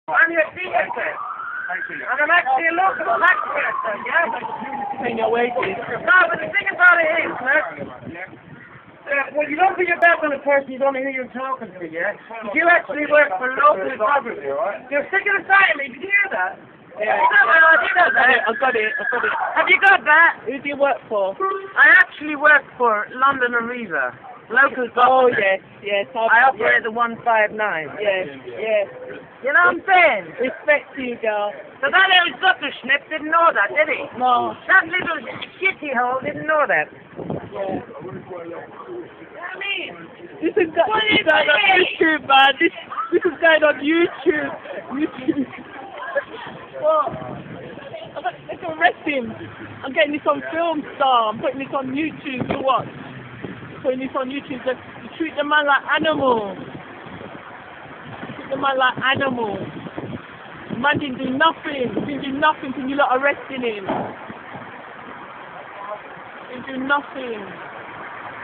brixton
Community Police officer Disrespecting a member of the puplic, not knowing who she was, the Community Police officer was so unprofessional & rude I dont know Y we have them & y r ppl paying Council tax 2 hav these officers on rd.  A short while l8r I found out she works 4 Arriva and Transport for London, she drives the 159 Bus.  Spoke 2 her briefly afterwards found out that she is also a gran mother of 6, she also got an apology 4rm da officer in question after demanding that he should apologise.